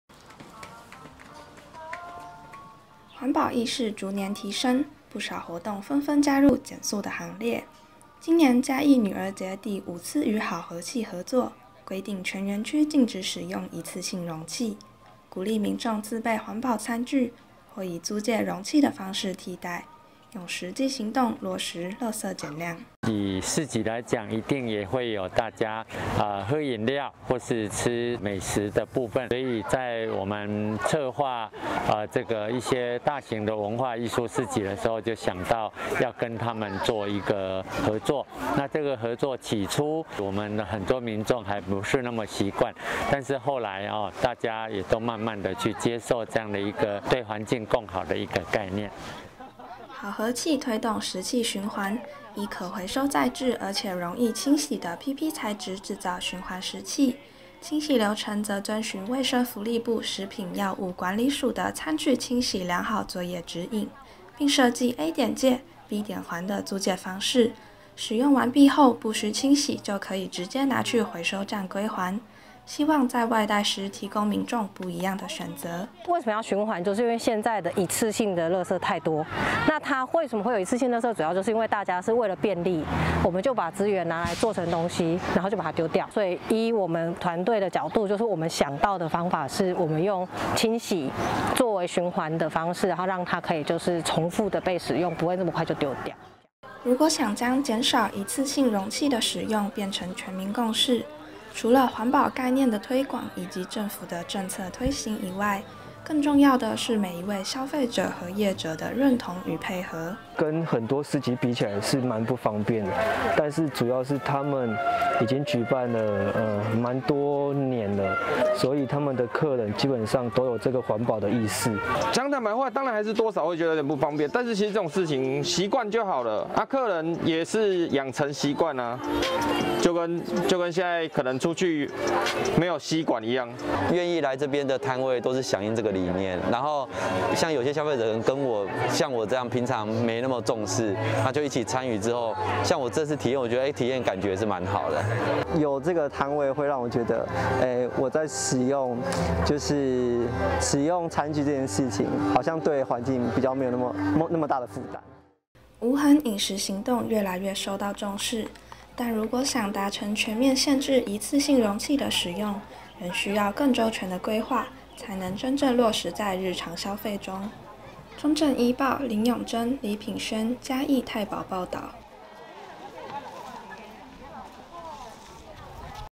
環保意識興起 食器循環再利用
【記者　林咏臻、李品萱／嘉義太保報導】

無痕飲食行動逐漸受到關注，今年嘉義女兒節再次與好盒器合作，全園區禁用一次性餐具，並提供食器租借的服務。食器租借的步驟簡單，後續清洗流程也遵循政府相關規定，提供民眾安全、便利的服務，希望藉由食器循環的推行，讓更多人將無痕飲食落實在日常生活中。